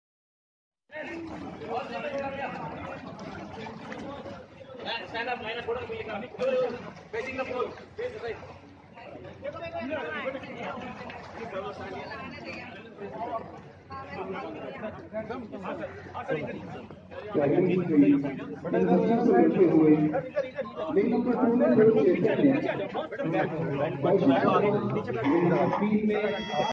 Swimming pool in Delhi government school
As a Supporter of @ArvindKejriwal
Such scenes from Delhi Government Schools, Makes me Proud !!
I care a damn about internal politics.
The very reason, AAP was made is being fulfilled by Honest Governance 👍